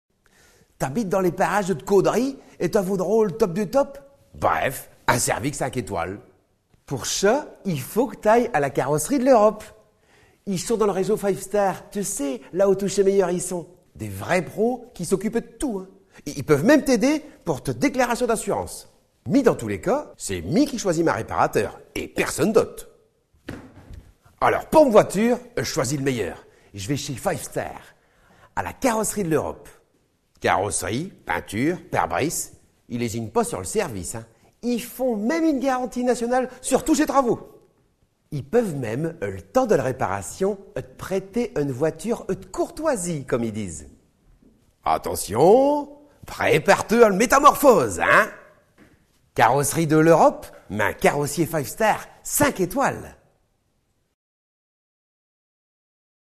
Tôlerie peinture auto Caudry
- Pas de bol, j'ai eu un accident!
- Moi je dis que tu as tout de même de la chance d'habiter la région de Cambrai ; je vais te conseiller un vrai pro en la matière !
La carrosserie de l'Europe, membre du réseau FIVE STAR ! Tôlerie peinture auto Caudry
Ta voiture va être comme neuve : Carrosserie, peinture, vitrage ...  Ils s'occupent de tout !
- Oui mais, je suis perdue avec tout ça, il faut que j'appelle mon assureur ??
- Fais comme tu veux mais saches que c'est toi qui choisit ton réparateur, et pour ça, je te conseille d'abord d'aller voir la , NS Auto qui va te donner les bons conseils !  En plus c'est top, il te met à disposition un véhicule de courtoisie le temps des réparations et, cerise sur le gâteau, il t'offre même une garantie nationale sur toutes ses prestations !
- Super, si j'ai un véhicule, j'vais pouvoir récupérer mes enfants à l'école ! Ça, c'est du service !
- La carrosserie de l'Europe,  « Votre carrossier FIVE STAR 5 étoiles » ! Tôlerie peinture auto à Caudry

"Aidez Sofian dans son combat contre l'ataxie telangiectasie"